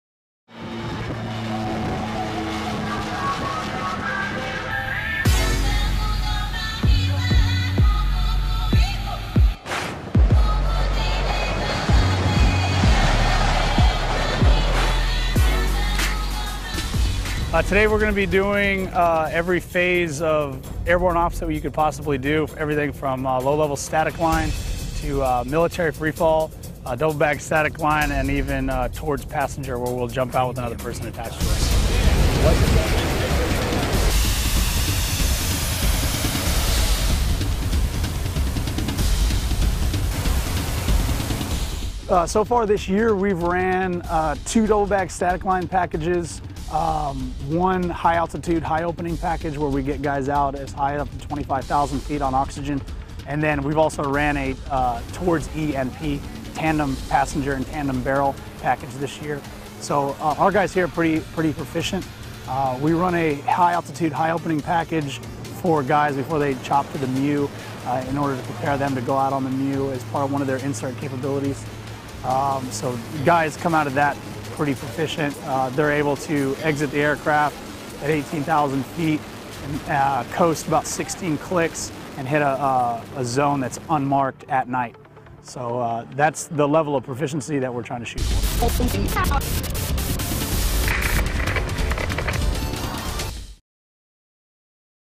Jump Ops | Reconnaissance Air Insertion
Marines with 2nd Reconnaissance Battalion conduct parachute operations at Camp Lejeune, North Carolina, Dec. 3, 2015. The training allows Marines to become technically and tactically proficient in several methods of insertion, giving them the capability to carry out their mission anywhere.

(U.S. Marine Corps video by Lance Cpl. Erick Galera)